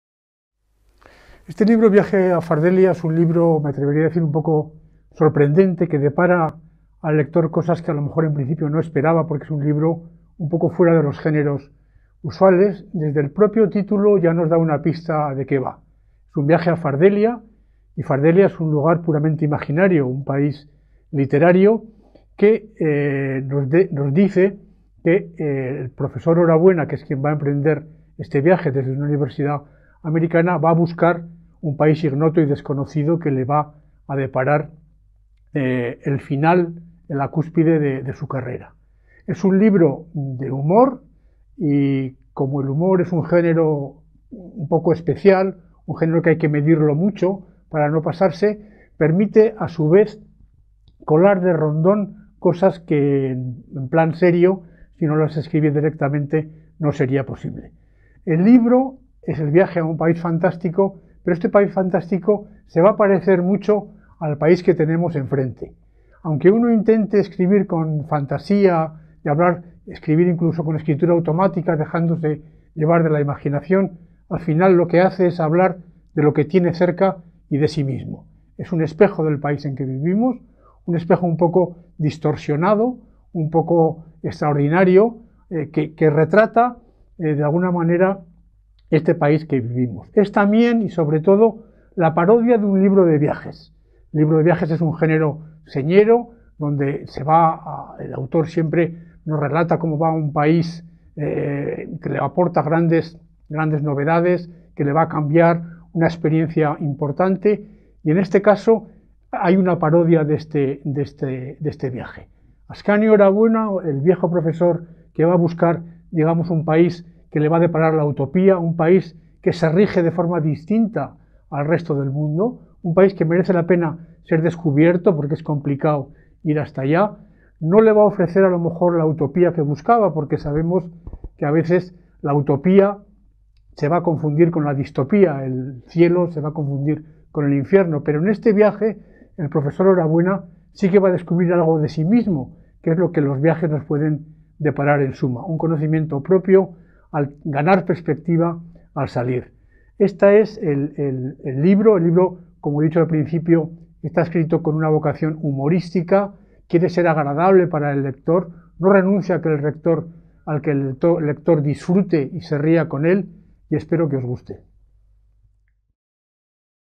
Viaje a Fardelia, de Pedro Charro Ayestarán, en M.A.R. Editor.
Novela fantástica y de humor protagonizada por Ascanio Orabuena, profesor de origen español que trabaja desde hace décadas en la Universidad de Polla Vieja (California). Rodeado de los recuerdos de toda una vida dedicada a investigar el comportamiento humano, decide emprender, antes de su jubilación, un último viaje a un país casi desconocido al que muy pocos han podido ir: Fardelia. Pero viajar a ese extraño y divertido país, según irá comprobando, resulta muy complicado.